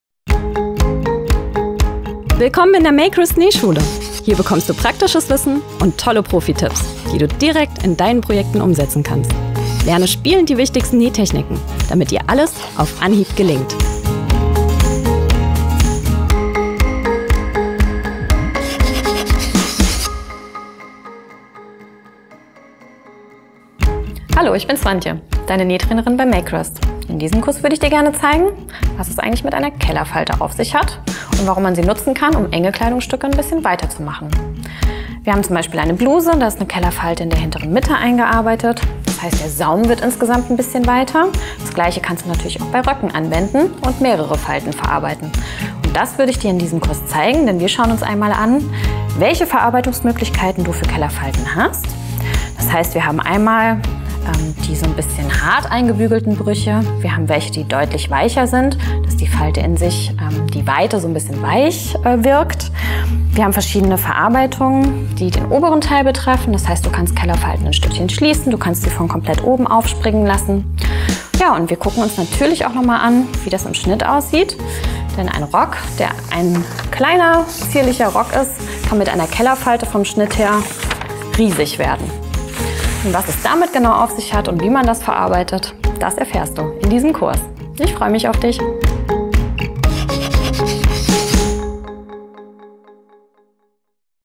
Trailer: Die Kellerfalte - 3 Varianten nähen
In diesem Kurs zeigt dir Swantje Wendt, was es mit einer Kellerfalte auf sich hat. Wie du damit unterschiedliche Kleidungstücke weitest und wie du drei unterschiedliche Varianten einer Kellerfalte nähst. Hier geht's zu Kurs